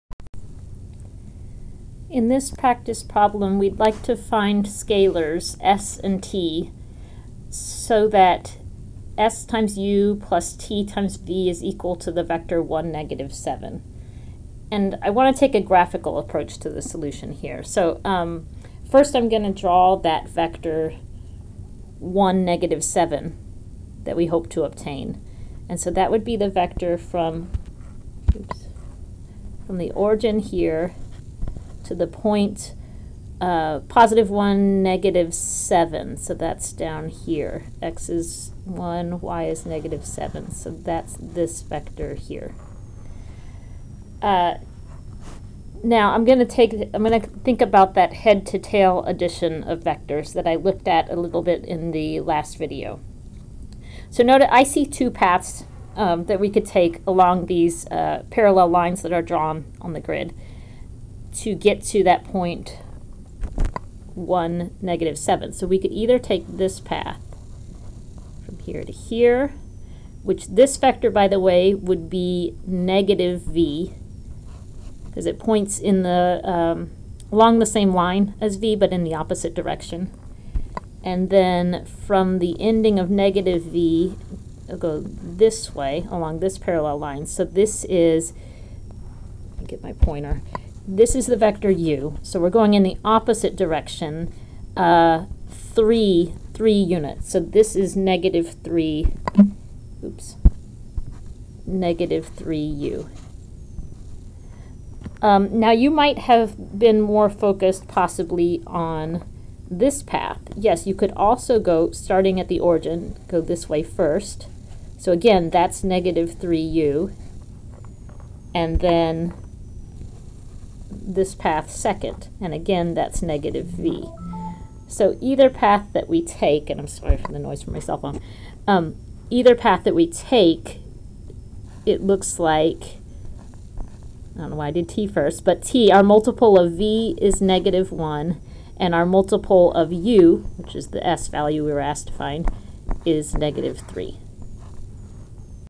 Finding a linear combination graphically
Writing a vector as a linear combination of two given vectors using graph.